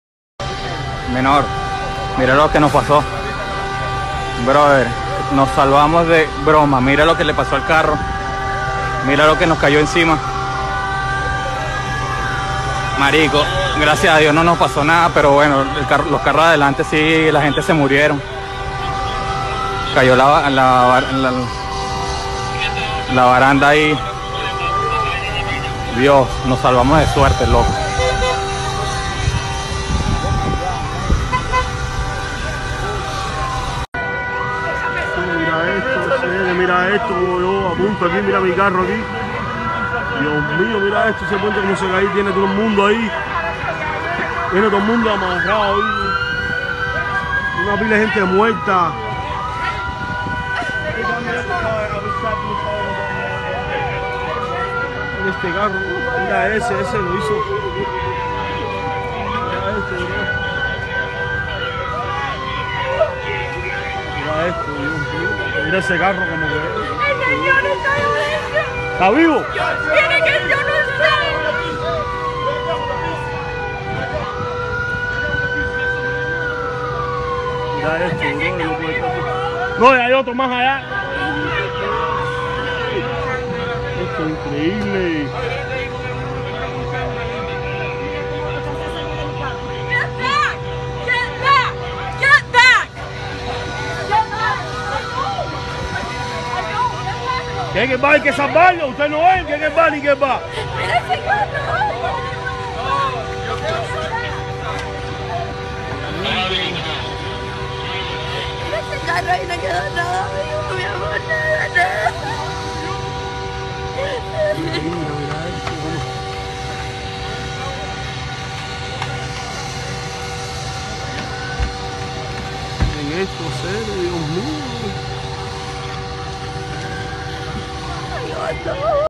Nicaragüense sobrevive a colapso de puente en Miami
Jesse Hamilton, un nicaragüense residente en Estados Unidos, nos envió el momento en el que colapsó el puente peatonal que se colapsó este jueves.